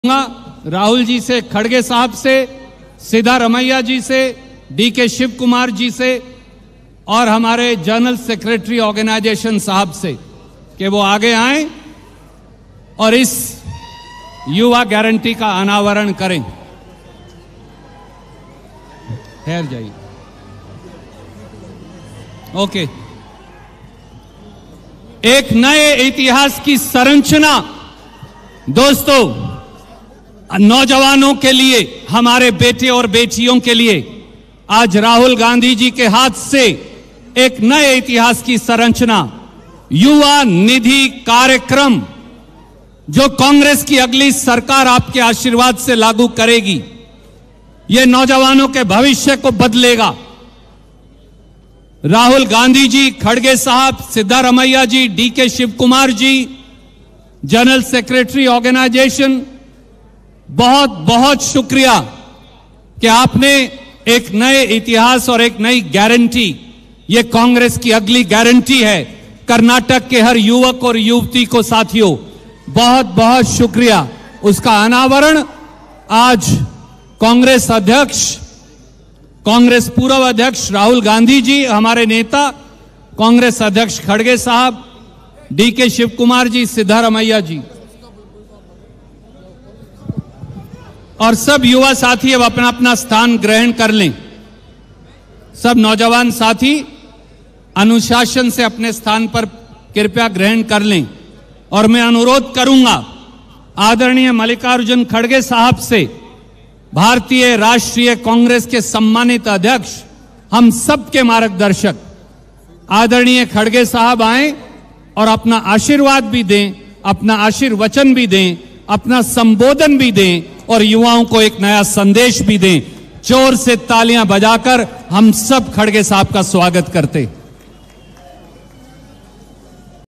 karnataka 4th guarantee : 3000 rs allowance for unemployed youth | rahul gandhi, siddaramiah, dks
Rahul Gandhi LIVE: Rahul Gandhi Belagavi Yuva Kranti Samavesha | DK Shivakumar, Siddaramaiah | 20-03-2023